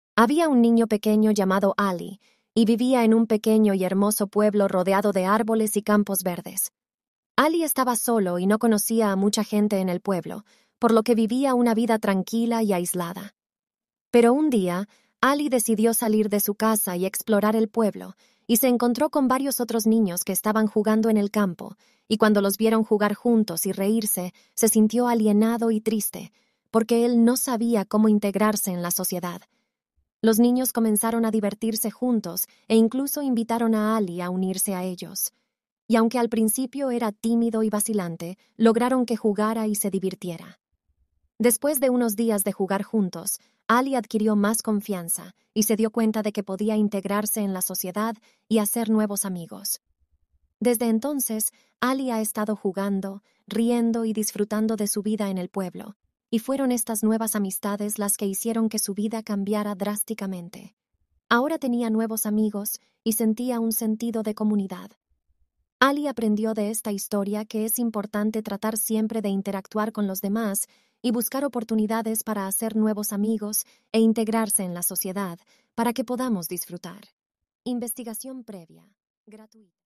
Había un niño pequeño llamado Ali, y vivía en un pequeño y hermoso pueblo rodeado de árboles y c
Había un niño pequeño llamado Ali, y vivía en un pequeño y hermoso pueblo rodeado de árboles y campos.